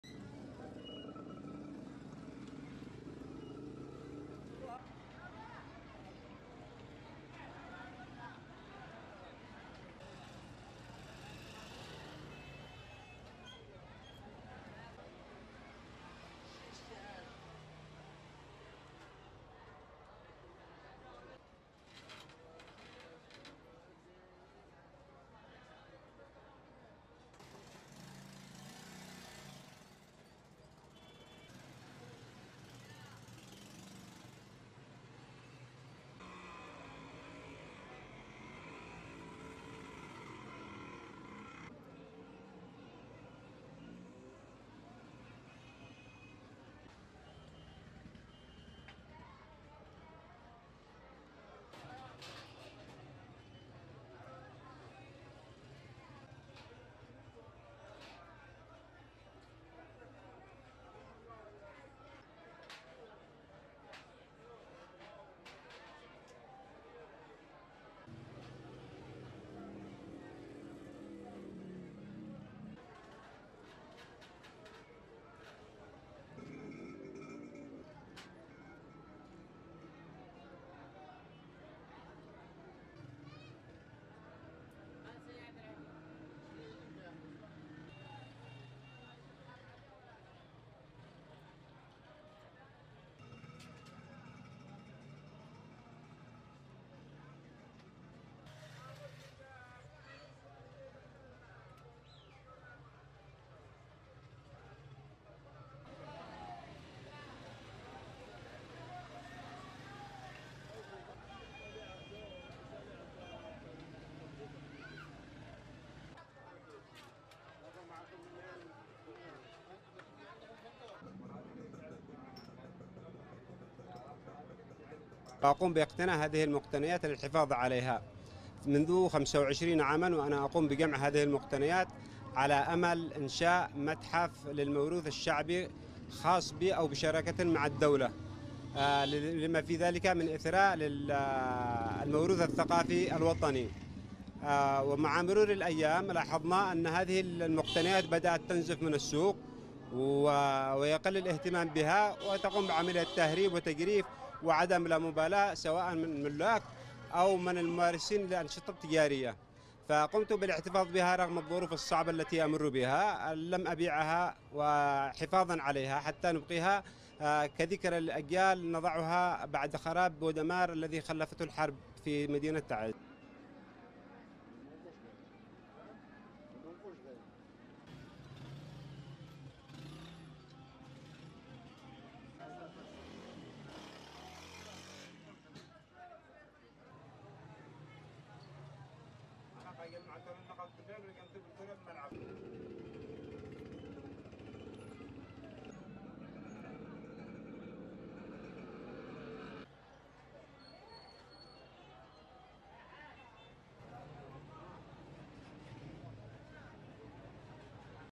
Yemeni Collector Aims to Convert Shop To Museum
Storyline
In war-torn Yemen, where artifacts and antiques are looted and ravaged, Abdullah Al Arifi insists on preserving and promoting the Yemeni cultural heritage by collecting  precious pieces. In a small shop, he showcases artifacts he has collected over 25 years. He now believes they are too precious to be sold ad aims to enrich the cultural legacy in Taiz, so coming generations can be introduced to their rich history

Shotlist (Audio: Arabic)
-Various shots of the shop
-Various shots of copper and silver antique pots
-Various shots of visitors
-SOUNDBITE (Abdullah Al Arifi- the shop’s owner):
I have been collecting those antiques for the last 25 years, hoping I would launch a museum to preserve and enrich our cultural legacy. Those precious pieces are no longer held in high esteem, as numerous pieces were smuggled and ravaged during the war. Owners of those artifacts don’t seem to care about their value. Therefore, I decided to keep them so coming generations can be introduced to this rich cultural heritage